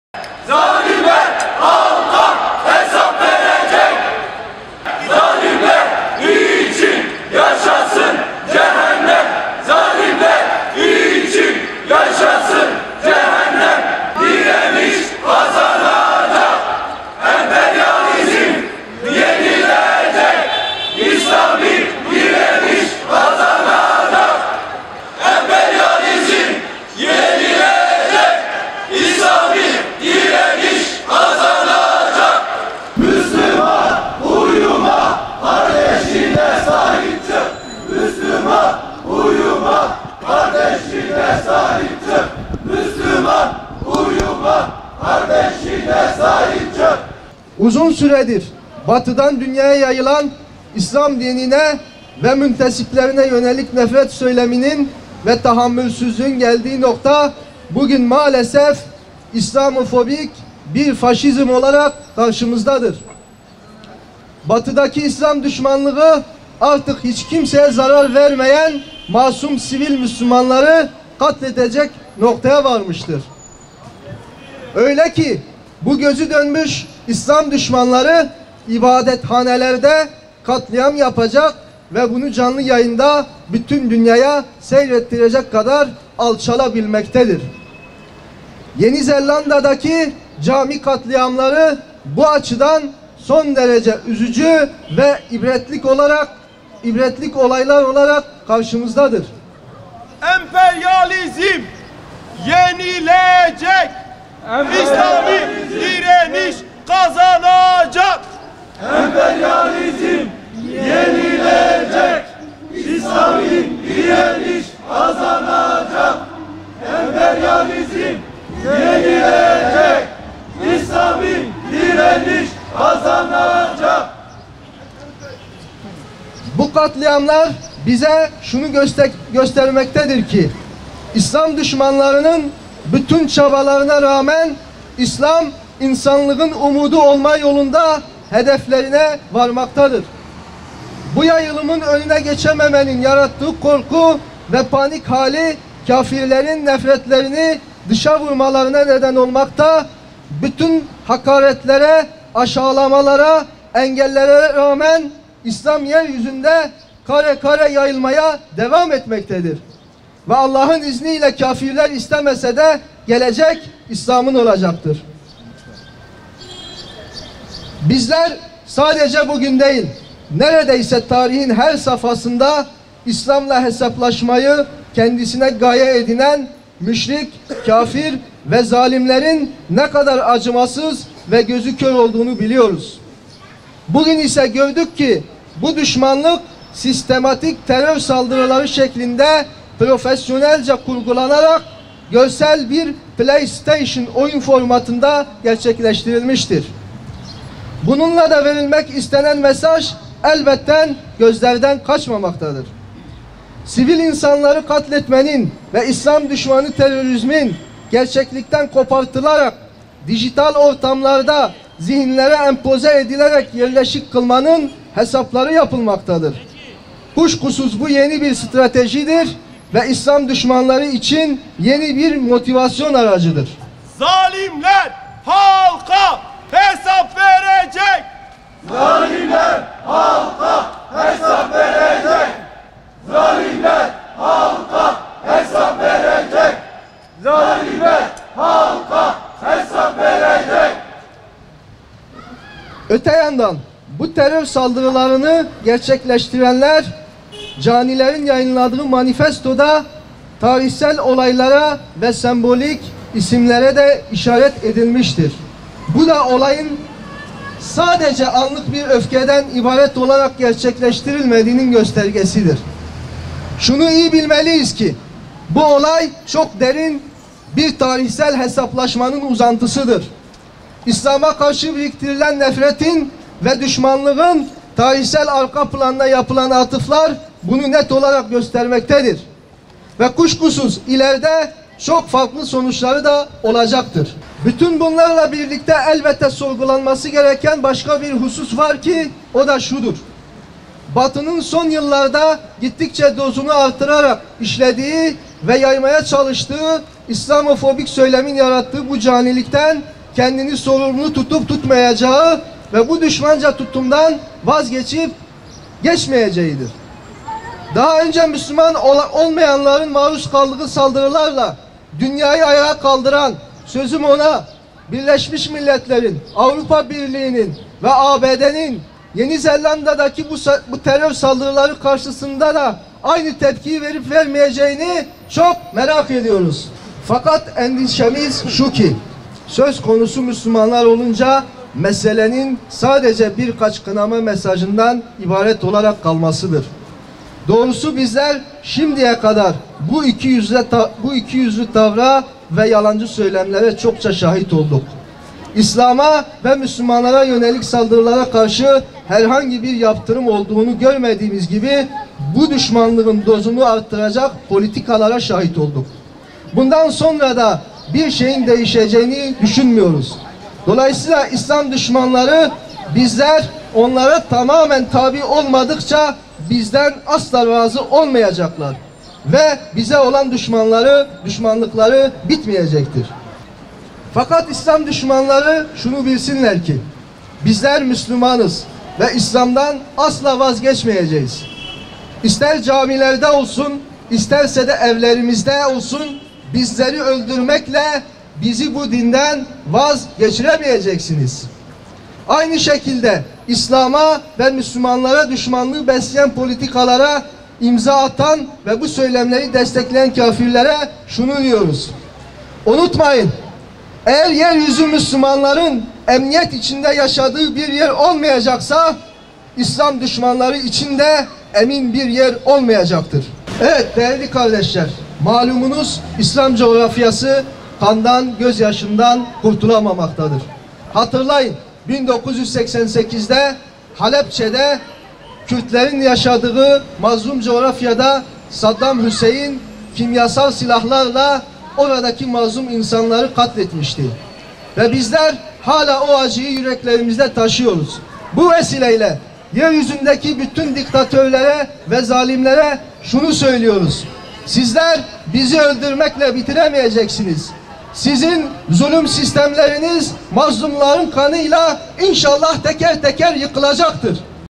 “İslam dini insanlığın umudu olma yolunda hedeflerine varıyor”
Diyarbakır'da, Yeni Zelanda'da gerçekleştirilen cami katliamına ilişkin düzenlenen basın açıklamasında, İslam düşmanlarının bütün çabalarına rağmen İslam dininin insanlığın umudu olma yolunda hedeflerine vardığı vurgulandı.
kaynak: İLKHA